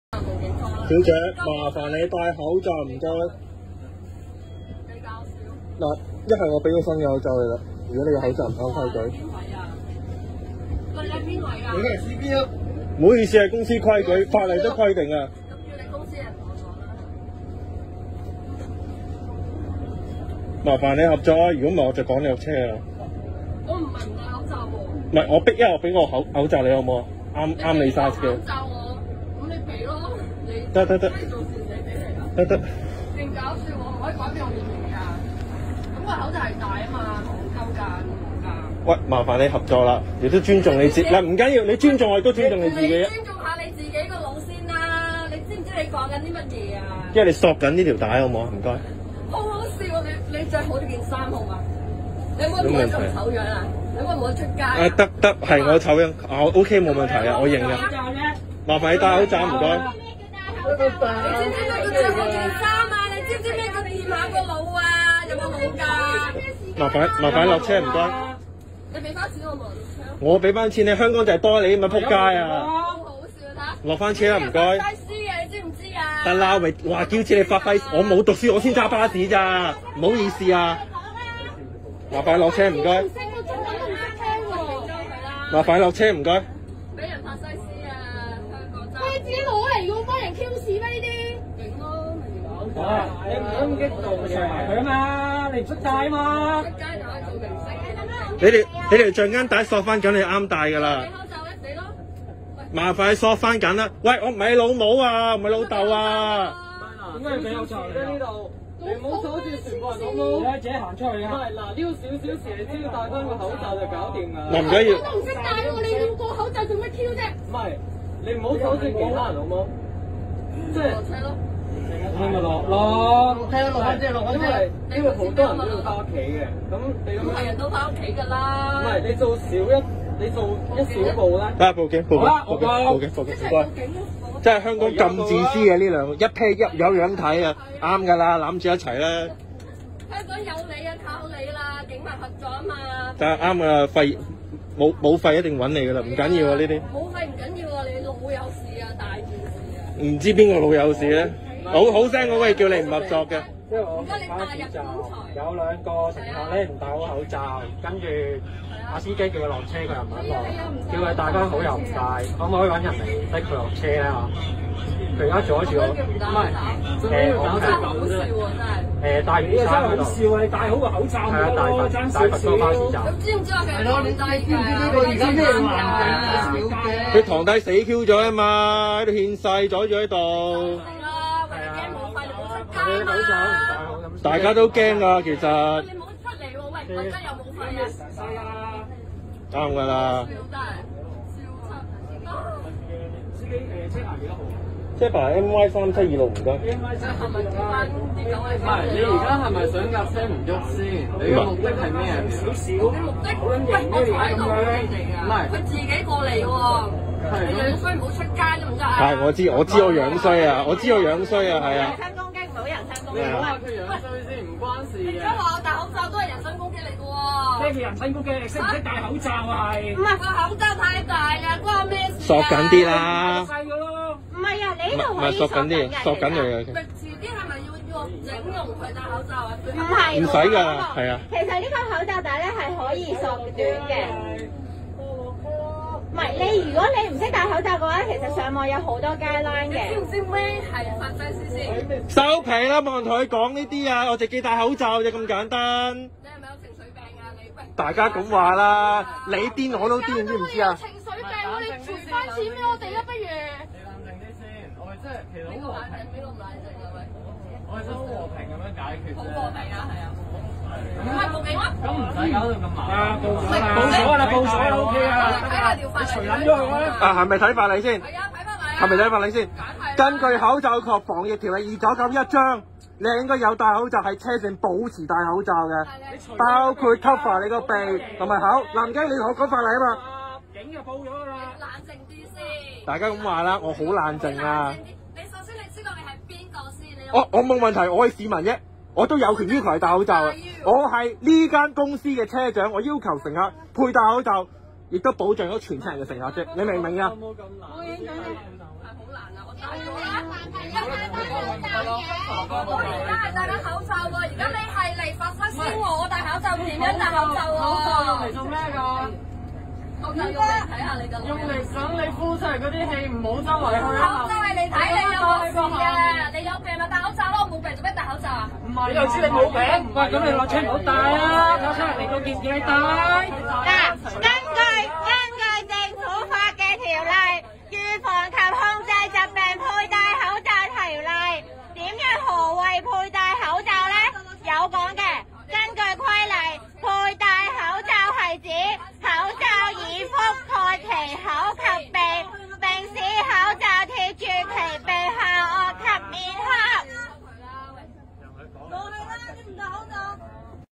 衰格男女昂坪搭巴士死都唔肯戴好口罩，唔肯落車仲諸多辯駁，罰款5000咎由自取
網上影片昂坪山頂,離乸晒罩
戴左成年口罩都唔識戴
冇咁大塊面唔好戴咁大個罩
戴慣A杯就唔好買G杯,托唔起架
提名及競逐2021年最仆街男女主角

網上流傳一段長達5分50秒的影片，片中男女結伴坐於巴士最後一排，二人的口罩僅覆蓋口部，並無遮掩鼻子。拍片的男子一度走近並勸喻女方：「小姐，麻煩你戴（好）口罩，唔該。一係我畀個新嘅口罩妳」，之後又表示自己是車長，正在根據公司規矩做事，要求戴好口罩，否則會趕二人落車。
怎料女方就用極不滿的語氣回應：「你係邊位？」、「我唔係唔戴口罩喎‥‥‥勁搞笑喎，咁我個口罩係戴（緊）吖嘛！」之後車長叫女子予以尊重，她卻突然情緒激動站起，又用帶人身攻擊的字眼反罵對方：「你尊重下你自己個腦先啦！你知唔知你自己講緊啲乜嘢呀？你著好件衫好冇呀？你可唔可以唔好咁醜樣呀？可唔可以唔好出街呀！」
車上多名乘客不滿女子的態度及行為，於是紛紛與車長合力指責她理應戴好口罩，不要阻礙其他乘客，又要求她落車等。而一直都一言不發的男子，卻竟突然嘲諷望向車長方向，並且拍掌。
其後車長表示報警處理，片中亦傳來男乘客疑似報警的對話。至於其他乘客就繼續「圍剿」該名女子，她就怒稱：「個口罩太大呀，關我咩事呀！遲啲係咪要整容去戴口罩呀我？」期間男方突然舉起中指，女方則指「睇法例啦」，車長及乘客隨即一人一句說出有關法例。直至片末，可見一名相信是女職員以強硬態度要求兩人落車。
片段引起網民熱烈討論，有人怒斥：「真想大巴大巴打埋去！」、「真係厚面皮同冇素質」。而警方就表示，昨晚約7時，接獲昂坪路一輛23號巴士的乘客報案，指一對情侶沒有戴好口罩。警員到場後，涉案29歲女子報稱血糖低不適，所以無戴好口罩，她之後被送往大澳診所治理，而案件則列作糾紛及有人不適處理。據了解，事件中目前沒有人被捕、被票控或被檢控。
頭條日報